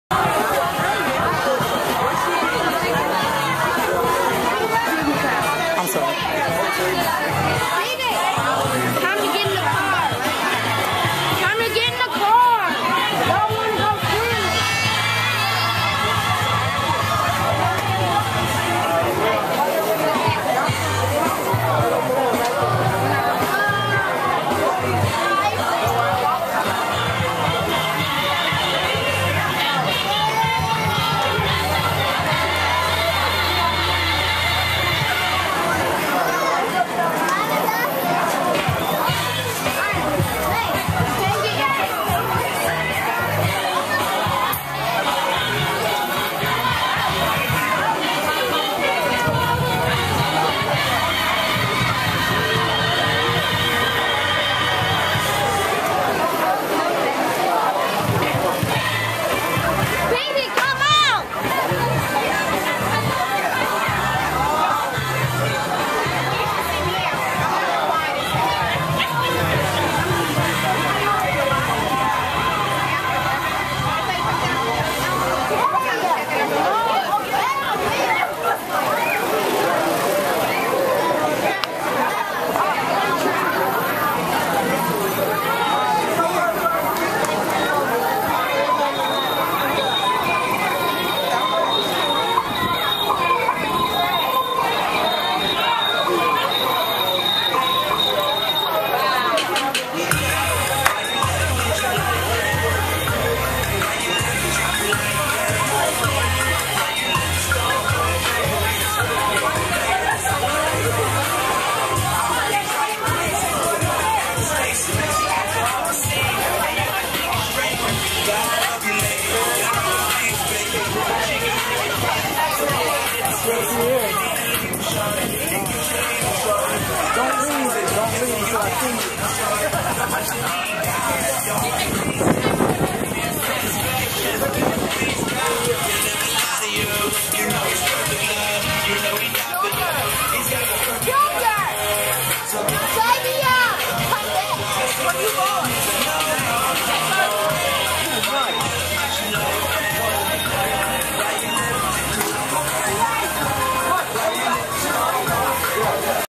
GCWC 2011 BACK PACK GIVE AWAY: FACE PAINTING
Face painting table was packed!